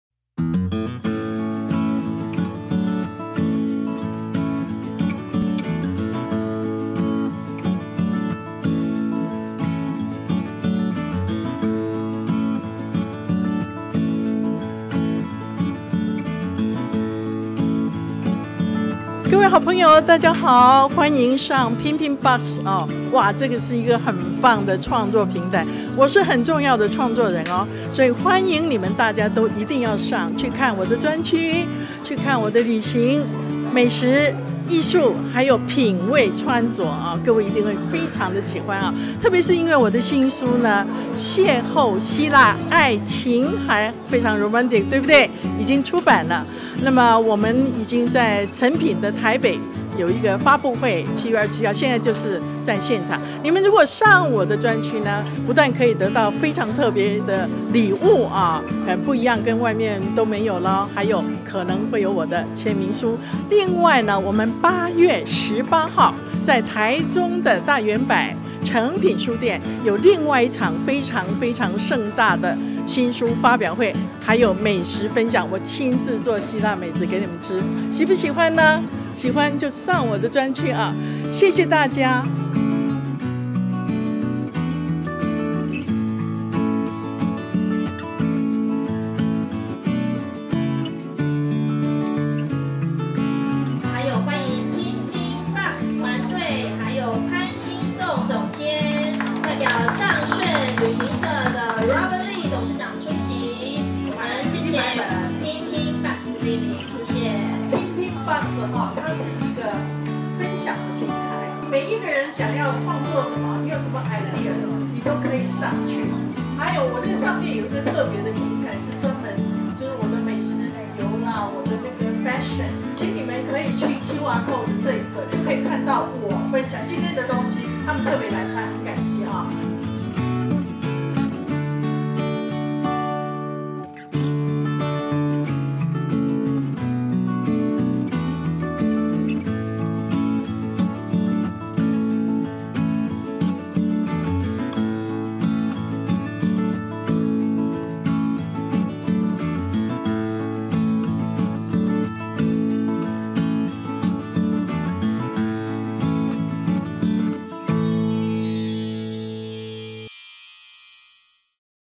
邂逅希臘愛情海！洪繡巒簽書會花絮 │ pinpinbox釘釘吧
洪繡巒老師的新書《邂逅希臘．愛情海》火熱發行中！
想嚐嚐美食旅遊達人的希臘美食嗎？
千萬別錯過最新穎的cooking show簽書會：
7/27 15:00~17:00 台北信義誠品
8/18 15:00~17:00 台中大遠百誠品